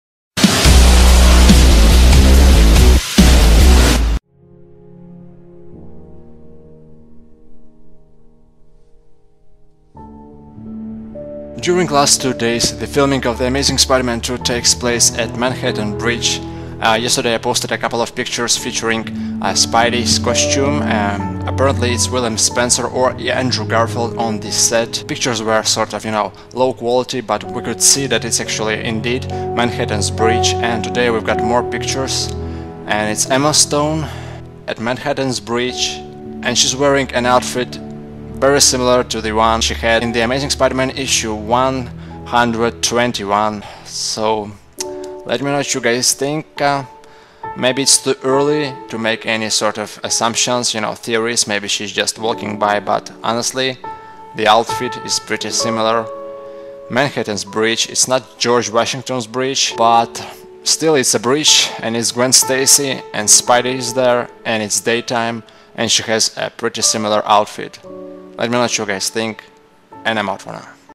Gwen Stacy At Manhattan Bridge On-Set Photos 'The Amazing Spider-Man 2'
The Night Gwen Stacy Died ► Filming at Manhattan Bridge, Between Manhattan and Brooklyn New York, NY 10002. All latest set pictures are available on my Facebook page ◄ My Twitter My Facebook